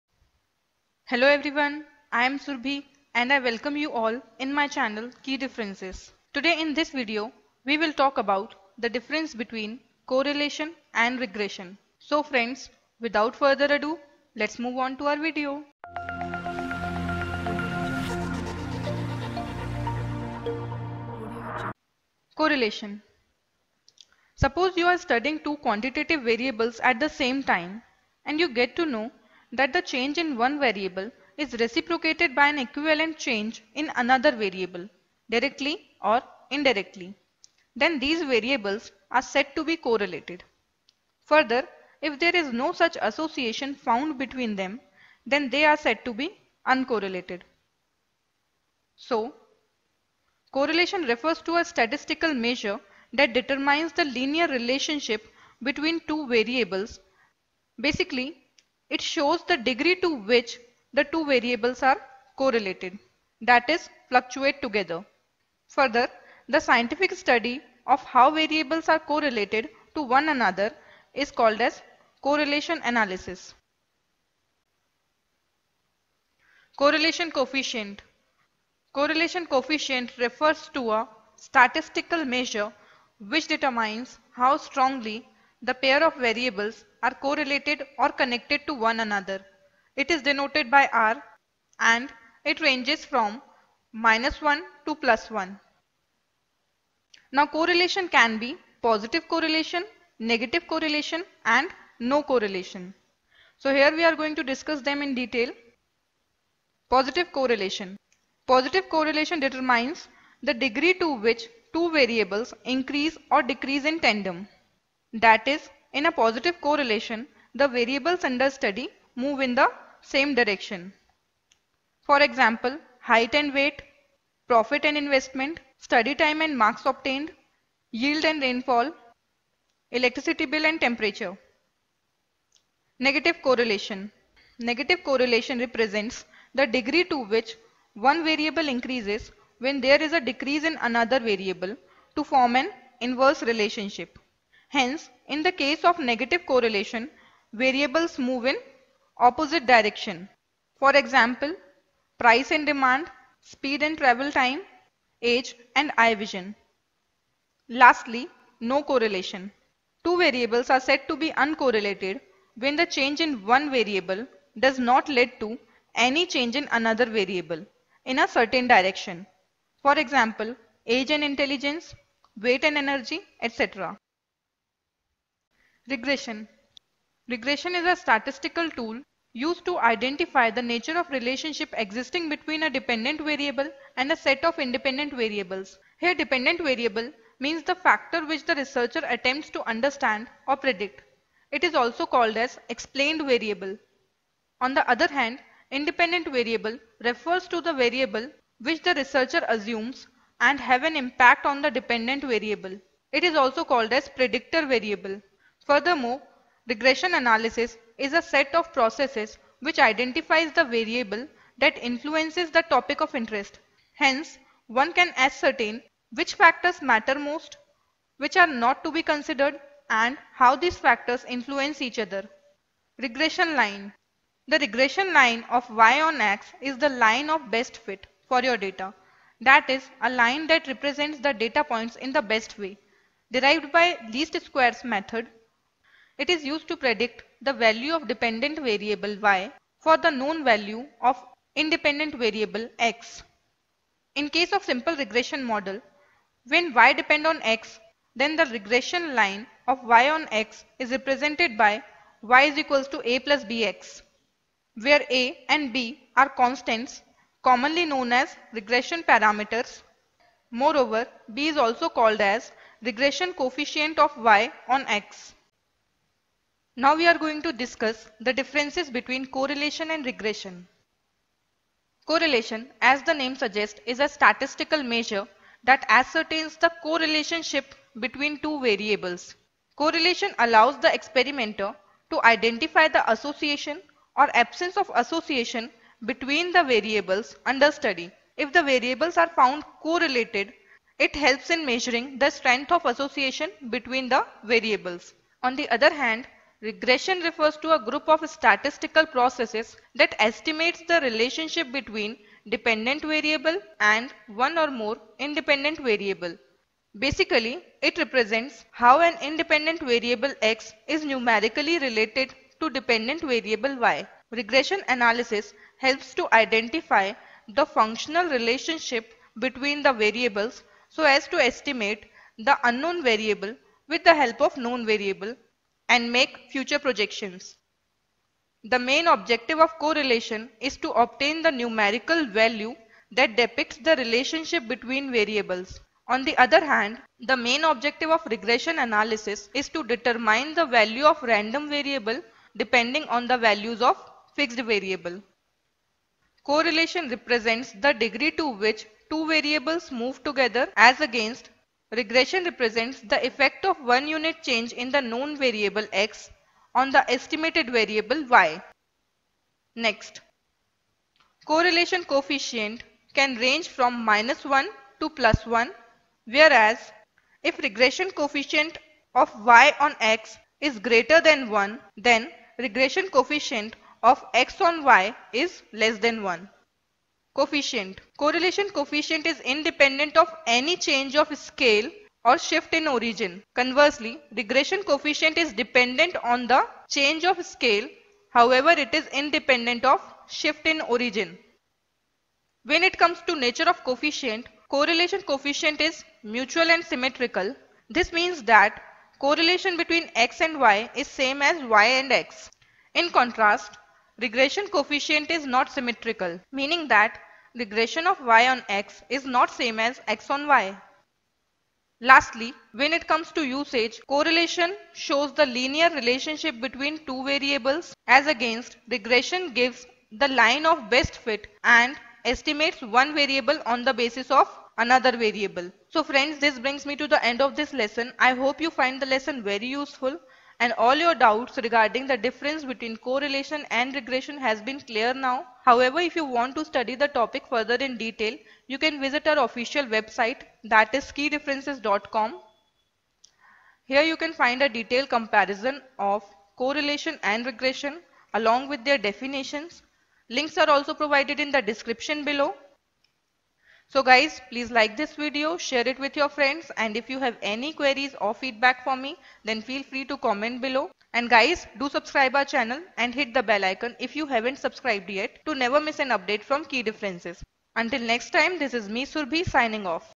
Correlation Vs Regression: Difference Between them with definition & Comparison Chart
In this session, we have explained the differences between Correlation and Regression. Also, the video talks about the meaning of the term 'correlation' and its types with examples. Further, in this video, the meaning of regression has been explained. Additionally, correlation coefficent and regression coefficent are also discussed.

At the end of this video, you will find the difference between correlation and regression in a detail.